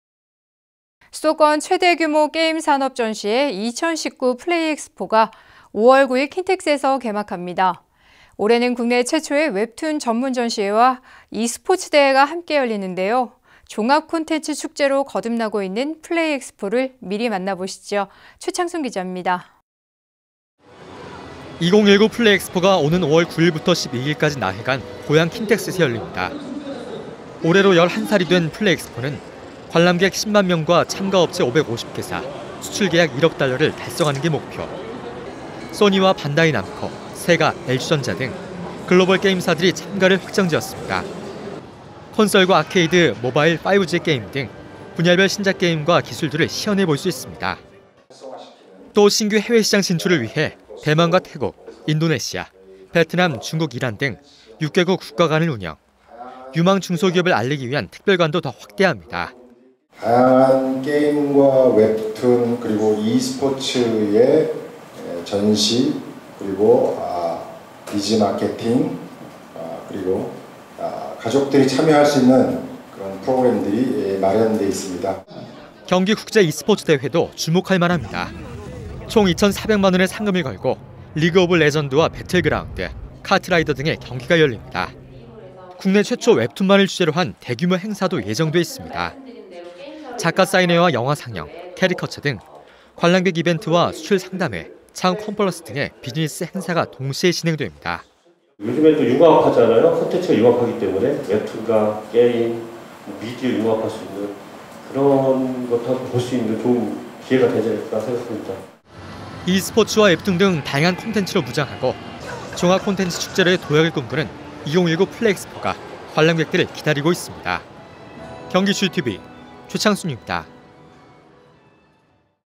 코앞으로 다가온 ‘2019 플레이엑스포’
[앵커멘트] 수도권 최대 규모 게임산업 전시회, 2019 플레이엑스포가 5월9일 킨텍스에서 개막합니다. 올해는 국내 최초의 웹툰 전문전시회와 e스포츠 대회가 함께 열리는데요. 종합 콘텐츠 축제로 거듭나고 있는 플레이엑스포를 미리 만나보시죠. 최창순 기자입니다.

[리포트]
2019 플레이엑스포가 오는 5월9일부터 12일까지 나흘간 고양 킨텍스에서 열립니다.

올해로 11살이 된 플레이엑스포는 관람객 10만 명과 참가업체 550개사, 수출계약 1억 달러를 달성하는 게 목표

소니와 반다이남코, 세가, LG전자 등 글로벌 게임사들이 참가를 확정 지었습니다.

콘솔과 아케이드, 모바일, 5G 게임 등 분야별 신작 게임과 기술들을 시연해 볼 수 있습니다.

또 신규 해외시장 진출을 위해 대만과 태국, 인도네시아, 베트남, 중국, 이란 등 6개국 국가관을 운영

유망 중소기업을 알리기 위한 특별관도 더 확대합니다.

[녹취] 오창희 / 경기콘텐츠진흥원장
“다양한 게임과 웹툰 그리고 e스포츠의 전시 그리고 비즈 마케팅 그리고 가족들이 참여할 수 있는 프로그램들이 마련돼 있습니다.”

경기 국제 e스포츠 대회도 주목할 만합니다.

총 2,400만 원의 상금을 걸고 리그 오브 레전드와 배틀 그라운드, 카트라이더 등의 경기가 열립니다.

국내 최초 웹툰만을 주제로 한 대규모 행사도 예정돼 있습니다.

작가 사인회와 영화 상영, 캐리커처 등 관람객 이벤트와 수출상담회, 창업 콘퍼런스 등의 비즈니스 행사가 동시에 진행됩니다.

[녹취] 안치권 / 경기도 콘텐츠산업과장
“요즘에는 융합하잖아요. 콘텐츠가 융합하기 때문에 웹툰과 게임, 미디어가 융합할 수 있는 그런 것을 볼 수 있는 좋은 기회가 되지 않을까 생각합니다.”

e스포츠와 웹툰 등 다양한 콘텐츠로 무장하고 종합 콘텐츠 축제로의 도약을 꿈꾸는 2019 플레이엑스포가 관람객들을 기다리고 있습니다.

영상취재 : 류민호, 영상편집 : 윤지성